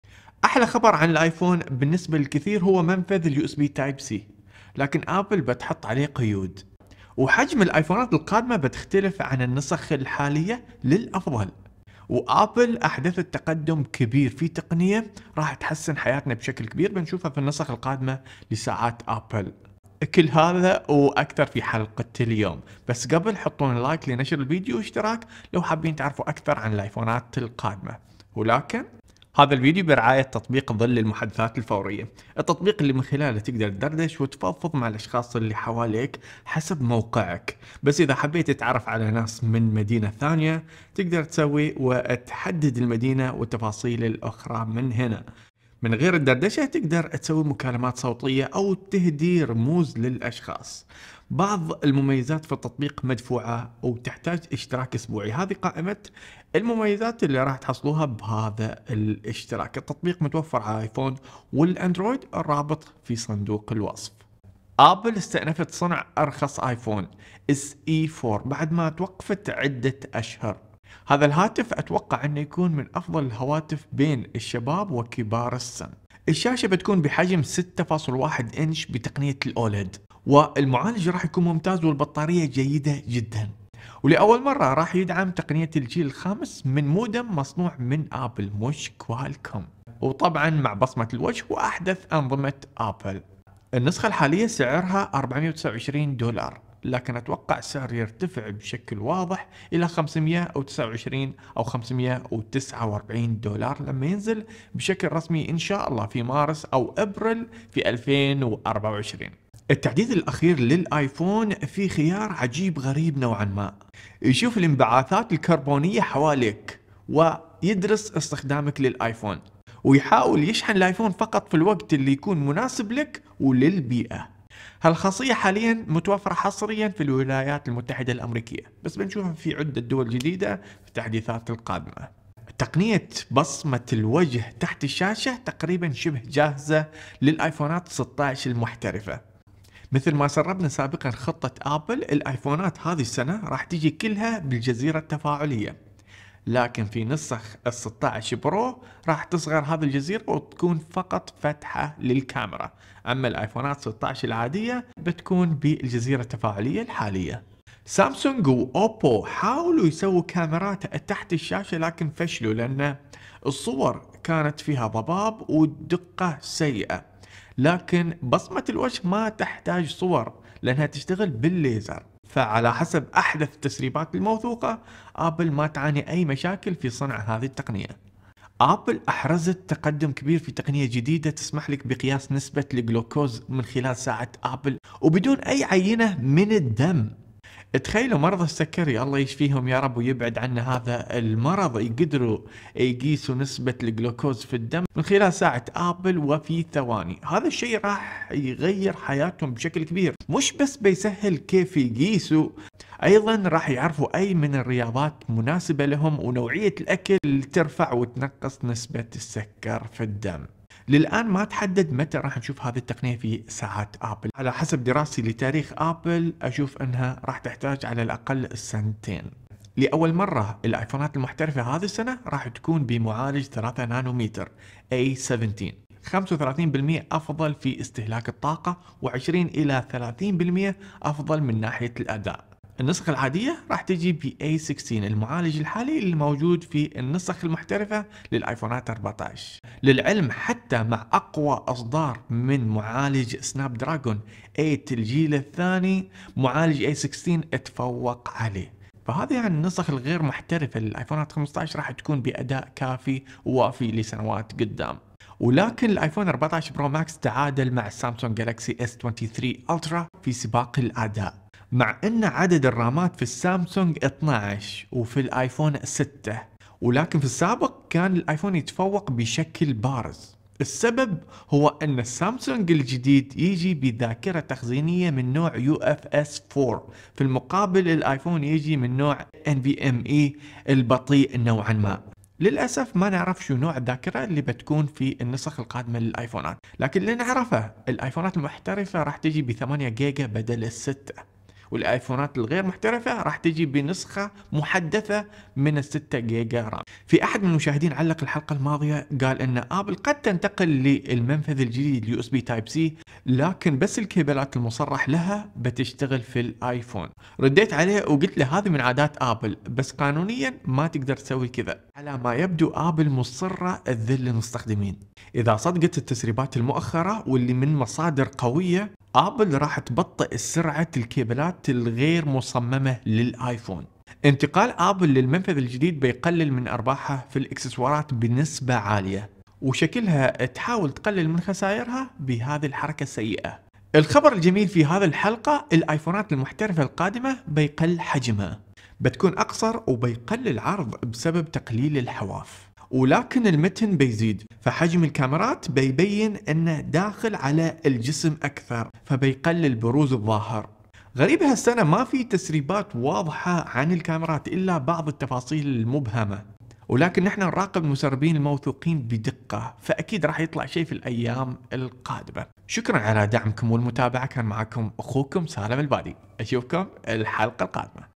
ايفون 15 برو Max | ابل تستعد لضرب سامسونج S23 الترا | iPhone 15 Pro Max
WhatsApp for iPhone now lets you easily create your own stickers from your photos

احلى خبر عن الايفون ١٥ بالنسبة للكثير هو منفذ التايب سي. لكن ابل بتحط عليه قيود.
وحجم الايفونات هالسنه بتختلف عن النسخه السابقة للاحسن!
وابل حققت تقدم كبير في تقنية بتغير حياتنا بنشوفها في النسخ القادمة من ساعة ابل!

بس قبل

ابل استأنفت صنع ارخص ايفون. اس اي ٤ بعد توقف لعدة اشهر. هذا الهاتف بيكون احد الهواتف المرغوبة للشباب و كبار السن.
شاشة بحجم ٦.١ انش اوليد. معالج ممتاز وبطارية جيده جدا.
دعم للجيل الخامس لاول مره من صنع ابل وليس كوالكوم.
وبصمة الوجه واحدث انظمة ابل.
النسخه الحالية سعرها ٤٢٩ دولار، اتوقع ارتفاع في سعر النسخه القادمة الي ٥٢٩$
بنشوفه ان شاء الله في شهر مارس او ابريل ٢٠٢٤

التحديث الاخير للايفون في خيار جديد عجيب غريب. يشوف الانبعاثات الكربونية فالمنطقة ويدرس استخدامك للايفون. ويحاول يشحن فقط فالوقت اللي يكون مناسب للبيئه للحد من التلوث الكربوني.
هالخاصية حاليا متوفره في الولايات المتحده وبنشوفها قريبا في دول اكثر.

تقنية بصمة الوجه تحت الشاشة شبه جاهزه في ايفونات ١٦ المحترفه. مثل ماسربنا سابقا خطة ابل.
ايفونات ١٥ كلها بتكون بالجزيرة التفاعلية، والسنه القادمة الجزيرة بتصغر ،بتصير فقط فتحة كامره
في نسخ البرو. وبتضل كبيرة النسخ العادية. سامسونج واوبو حاولوا يصنعوا كامره تحت الشاشة بس الصور كانت فيها ضباب ومشاكل. بس لانه بصمة الوجه تحتاج ليزر مش صوره ابل بتقدر تخليها تحت الشاشة.

ــــــــــــــ

ابل احرزت تقدم كبير في تقنية قياس نسبة الجلوكوز فالدم عن طريق ساعة ابل. تخيلوا مرضى السكرى الله يشفيهم. بيقدروا يعرفوا نسبة السكر فالدم من الساعه مباشره بدون الحاجه لاخذ عينه من الدم.
هالشي مب بس بيسهل قياس الجلوكوز، بيساعد المرضى في تحديد الرياضات اللي تحسن النسبه وانواع الاكل المفيد والمضر في حميتهم. للان ماتحدد اذا بنشوفها الساعه القادمه من ابل. بس اتوقع نحتاج سنتين.

لاول مره فالايفون النسخ ١٥ المحترفة بتكون بمعالج ثلاثة نانو متر A17 .
٣٥٪ افضل من ناحية استهلاك الطاقة واداء افضل بين ال٢٠-٣٠٪
النسخ العادية بتكون بمعالجات A16 اللي موجوده فالنسخ المحترفه الحالية
للعلم حتى مع اخر اصدرا لاقوى معالج في هواتف الاندرويد الرائده سناب دراجون ٨ الجيل الثاني
ابل بعدها تفوقت بال A16

الايفون ١٤ برو ماكس تعادل مع سامسونج جالاكسي اس ٢٣ الترا في سباق الاداء
مع انه عدد الرامات في السامسونج ١٢ والايفون ٦. لكن فالسابق الايفون كان يتفوق بفارق كبير.

سبب التعادل هو سرعة الذاكره التخزينية فالالترا يو اف اس ٤ اسرع بكثر عن الذاكره فالايفون من نوع NVMe

للاسف مانعرف شو نوع الذاكره اللي بيكون في نسخ ال١٥ بس اللي نعرفه هو زيادة عدد الرام من ٦ الي ٨ في النسخ المحترفه
و العادية بتحصل ترقيه في سرعه الرام بس بتضل ب ٦ جيجا.

في احد الاشخاص علق فالفيديو السابق انه ابل قد تنتقل للمنفذ الجديد بس الكيبلات من الشركات الثانية ما بتشتغل!

رديت عليه وقلتله هذي من عادات ابل ولكن مابتقدر تسوي كذا قانونيا!

ولكن على ما يبدوا
ابل مصره تذل المستخدمين اذا صدق هذا التسريب واللي من مصادر قوية

الكيبل اللي ما يكون مخصص للايفون بيكون بسرعات نقل ابطأ.

انتقال ابل للمنفذ الجديد بيقلل من ارباحها في الاكسسوارات بنسبه عالية.

وشكلها تحاول تعوض خسايرها بالحركة السيئه

الخبر الجميل في هذي الحلقة الايفونات المحترفه القادمة بتصغر ابعادها
بتكون اقصر وبيقل العرض. بسبب تقليل الحواف. لكن المتن بيزيد.
 فحجم الكامرات بيبين انه داخل عالجسم اكثر فليقل البروز الظاهر.

ــــــــــــــ

غريبه هالسنه ما في تسريبات واضحه عن الكامرات الا بعض التفاصيل المبهمه.

ولكن نحنا نراقب المسربين الموثوقين بدقه فاكيد بيطلع شيء خلال الايام القادم.

شكرا على دعكم والمتابعه. كان معاكم اخوكم سالم البادي. اشوفكم الحلقه القادمة.

لتكون انت المليونير التالي
رابط قناتنا المتخصصة في ادارة واستثمار المال والوقت